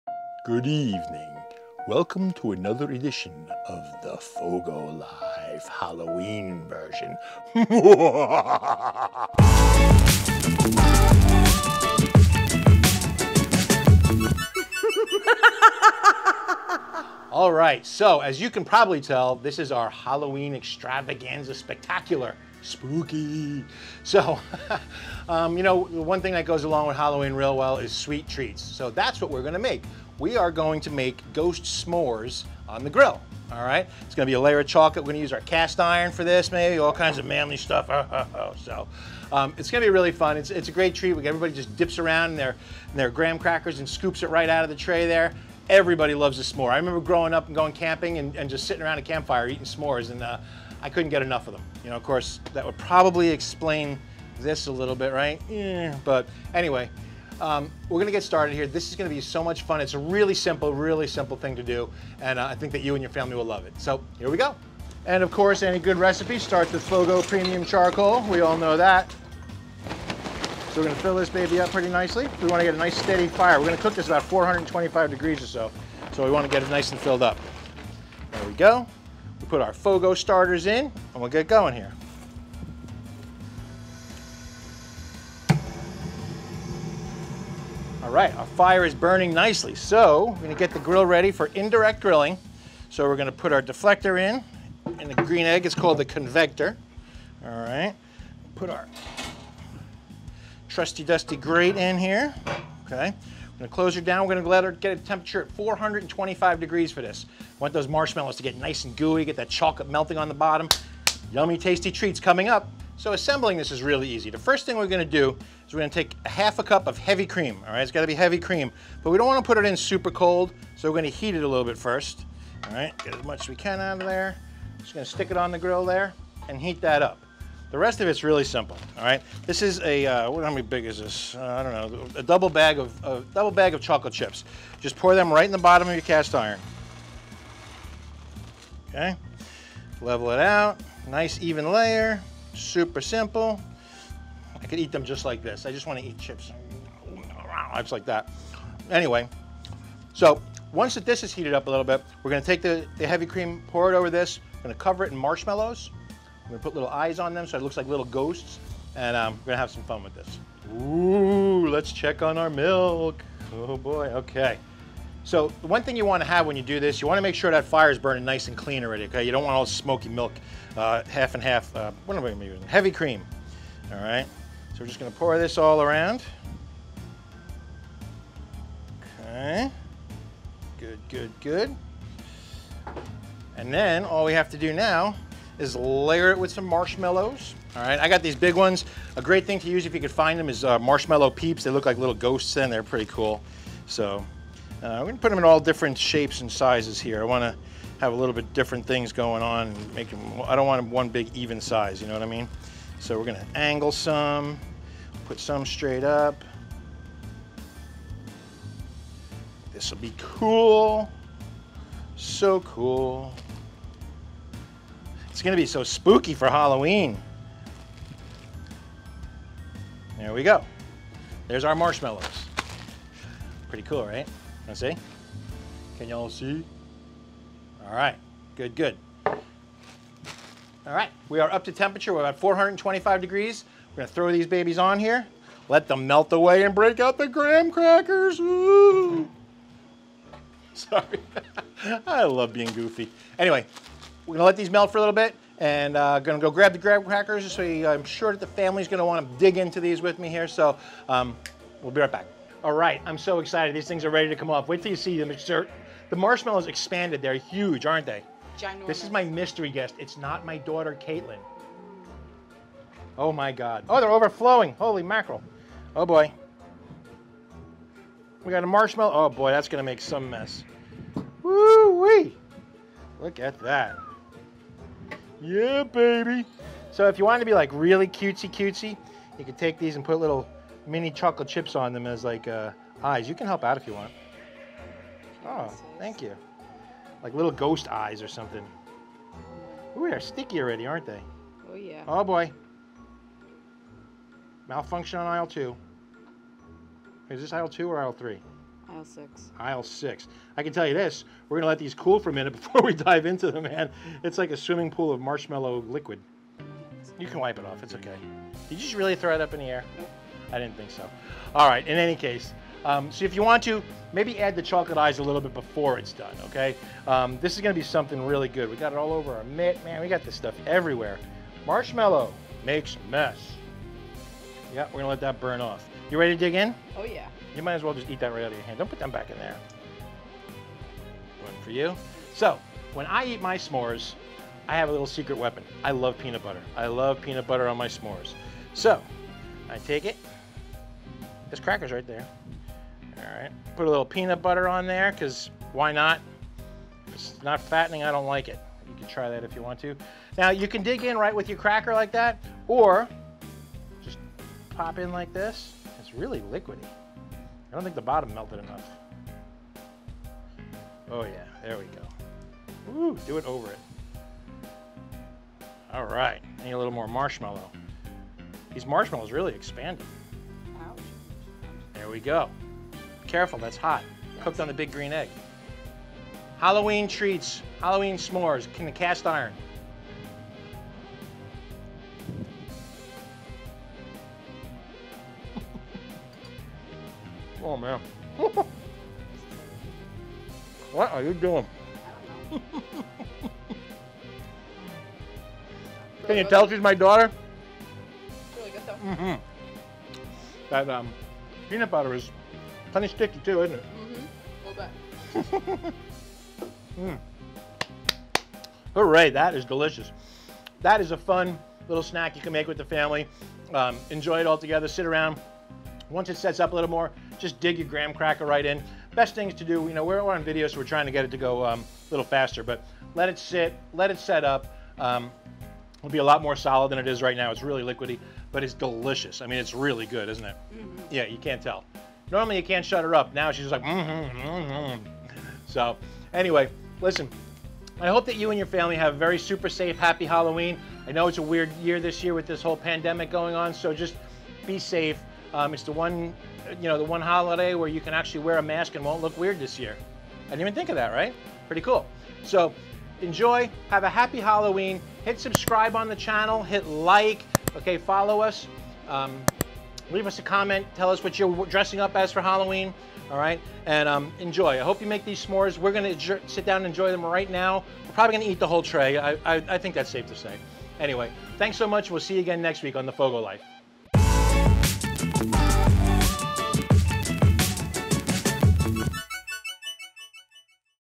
S'mores on the Big Green Egg | Grilled Dessert
My kids LOVE Halloween and because this year we will have to do things a bit differently, we want to make it extra special! Thankfully Ron came up with this great idea to do on the grill. I’m sure my kids will get a kick with these ghostly cast iron s’mores...a combination of crunchy graham crackers, melty chocolate and ooey gooey marshmallows.

If you’re looking to start cooking, have FOGO CHARCOAL delivered directly to your backyard!